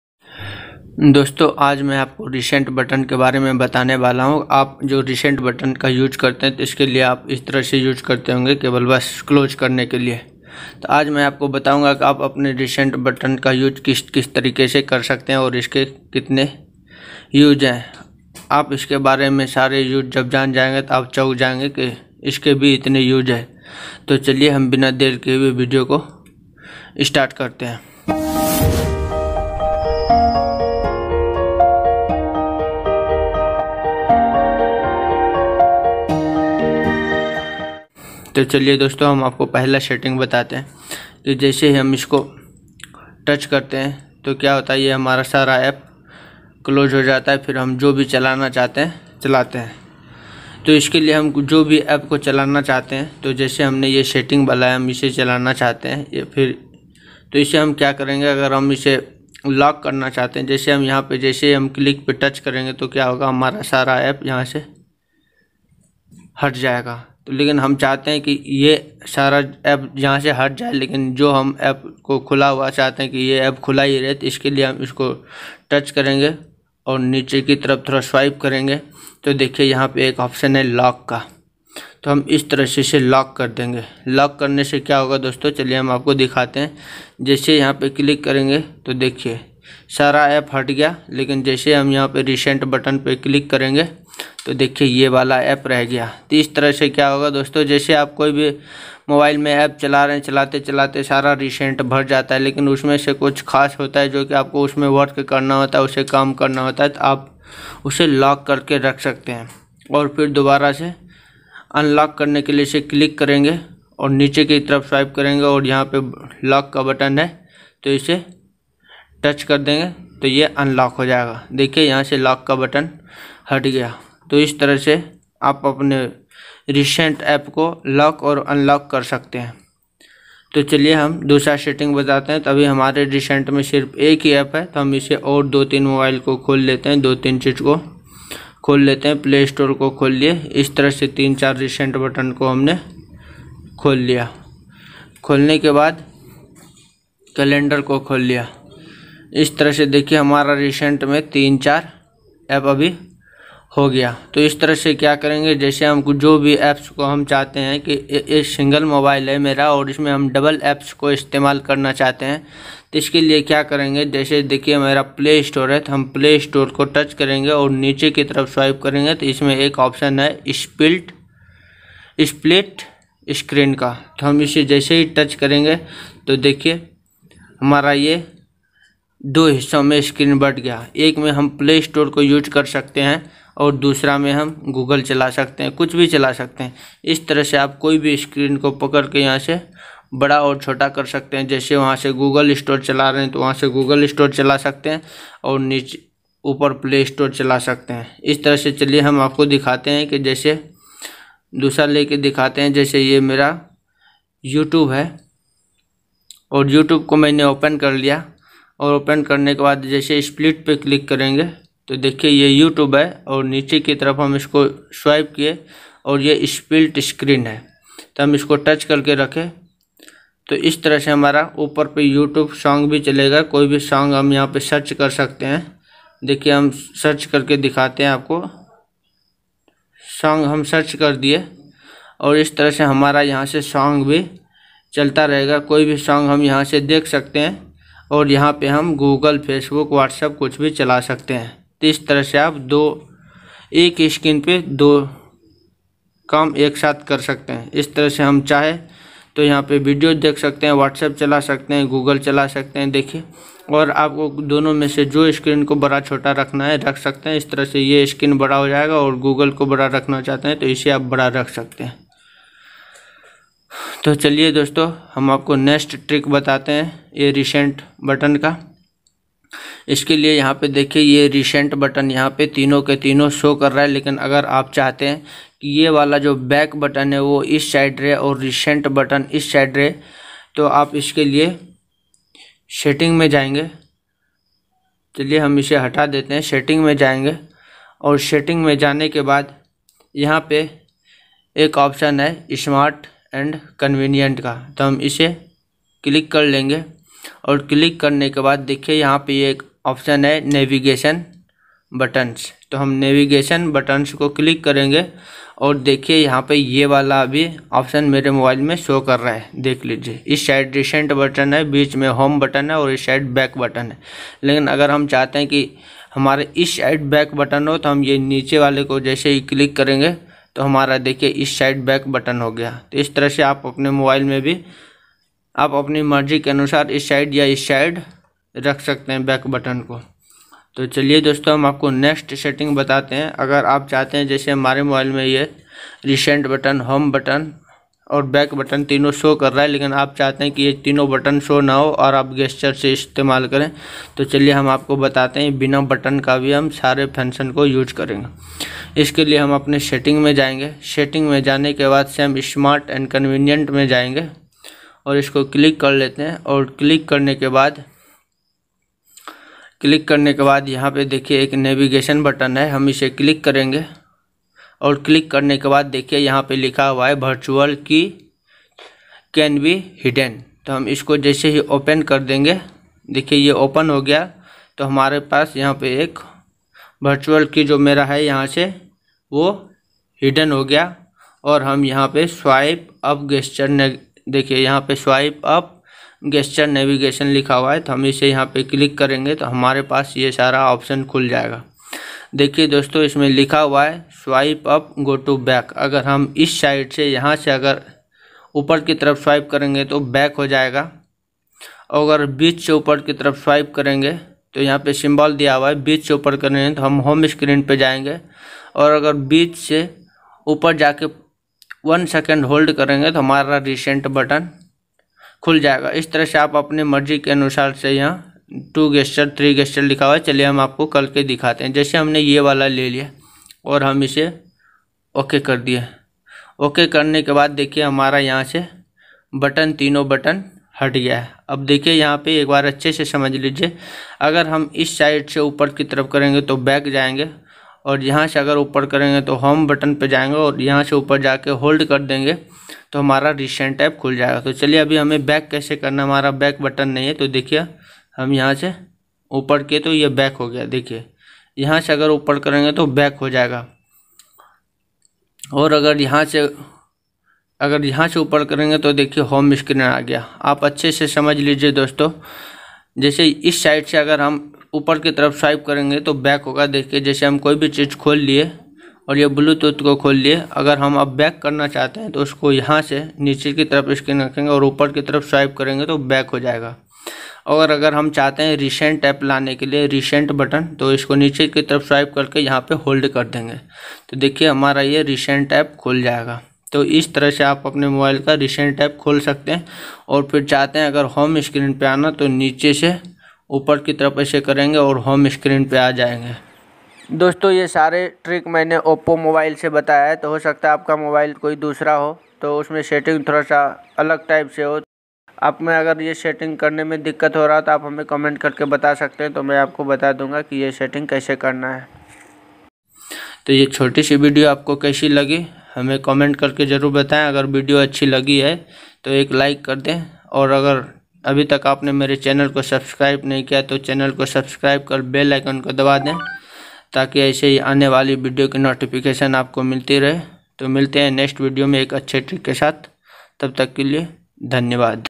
How to use reset button in Hindi recent button ka use kaise karen
Hello friends aaj ke video mein ham aapko recent button ke sare trick k bare mein pure aasan trikon se bataenge is video ko dekhne ke bad recent button ke jitne bhi use hote Hain vah sare trick aapko aasani se samajh jaenge so ok doston thank you bye bye take care